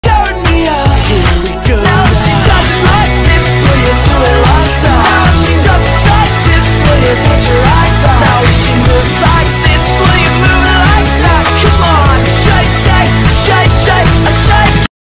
Elsa teaches Anna how to dance
Elsa and Anna love to dance… but maybe Anna needs more lessons.